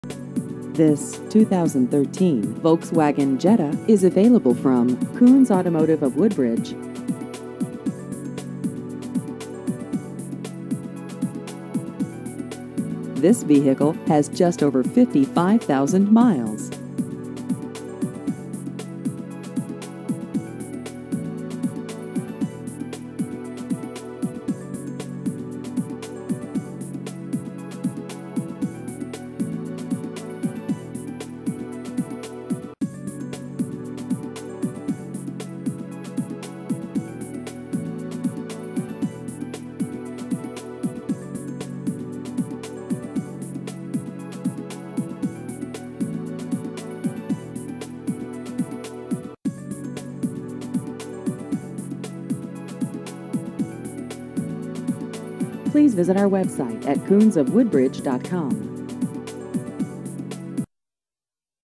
2013 Volkswagen Jetta Used Cars Woodbridge VA
This 2013 Volkswagen Jetta is available from Koons Automotive of Woodbridge.